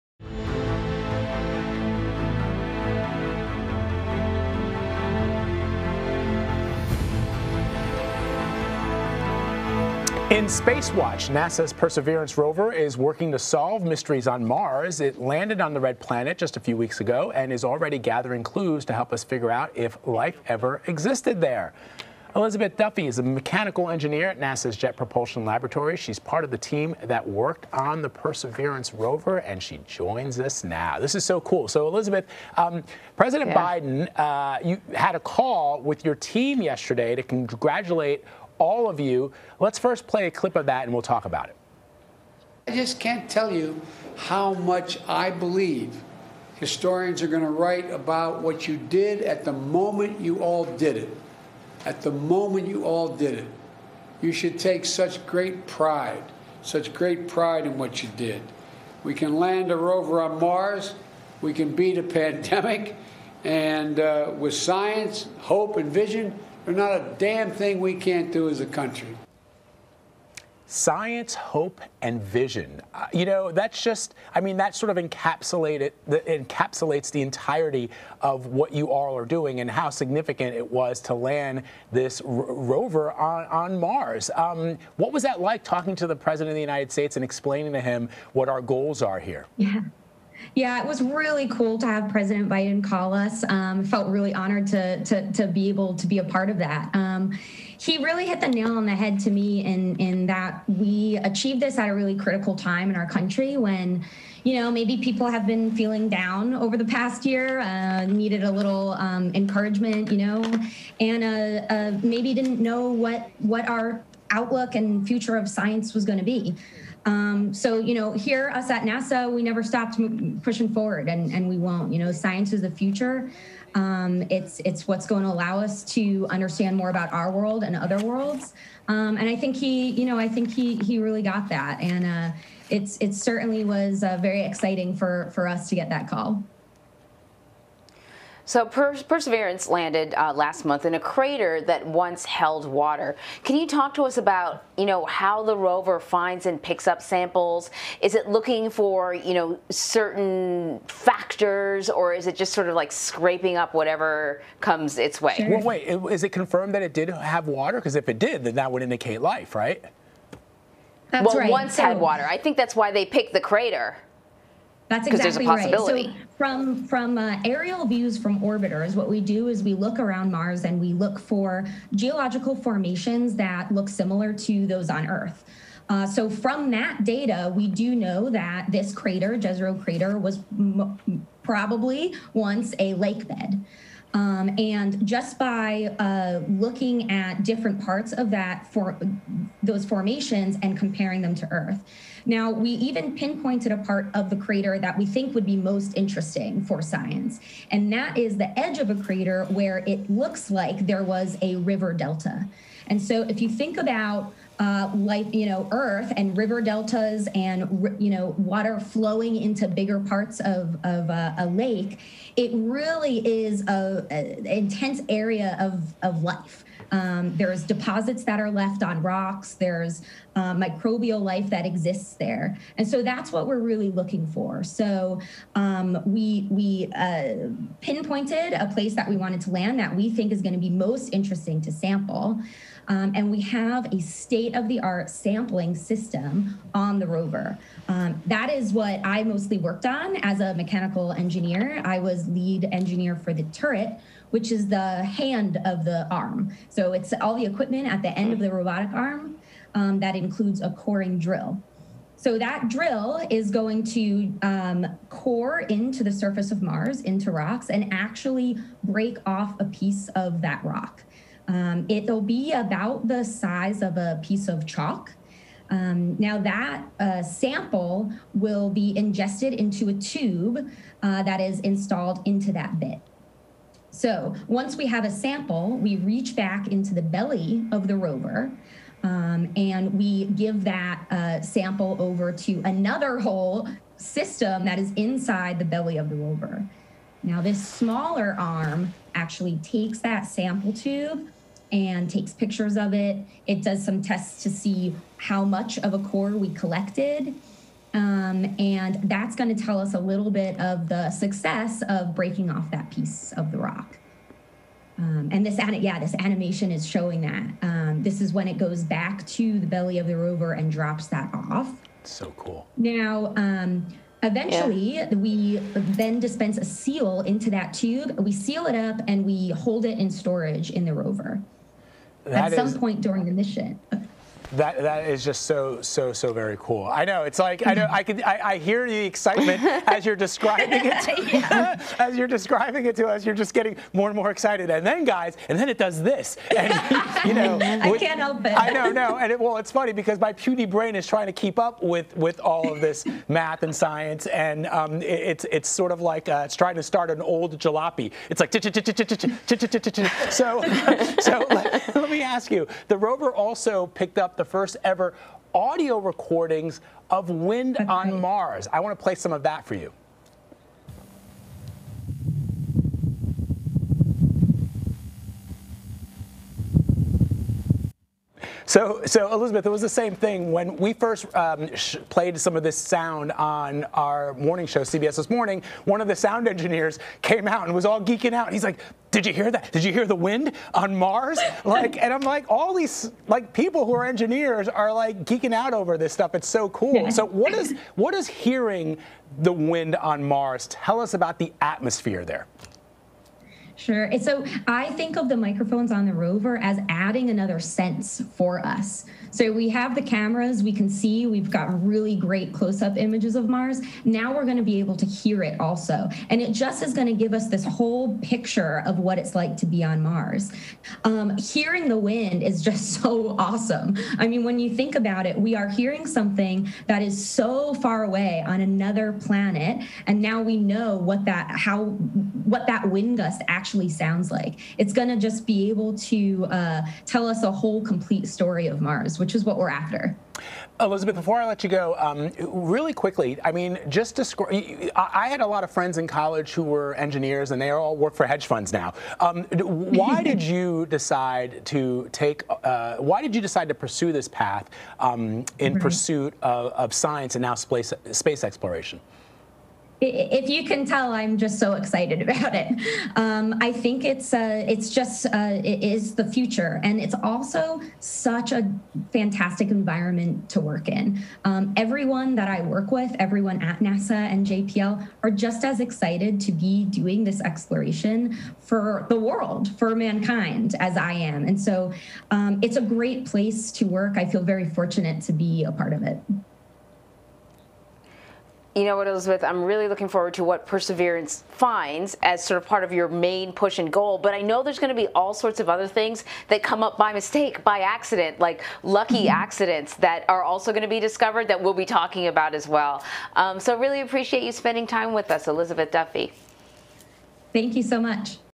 NASA engineer on call with President Biden, Mars mission
President Joe Biden called the team behind the NASA Perseverance rover to congratulate them Thursday on a successful landing on Mars. Elizabeth Duffy, a mechanical engineer with NASA's Jet Propulsion Laboratory who worked on the project's sample collecting system, spoke with Anne-Marie Green and Vladimir Duthiers on CBSN about getting accolades from the president, the amazing discoveries made on the red planet so far and what's to come.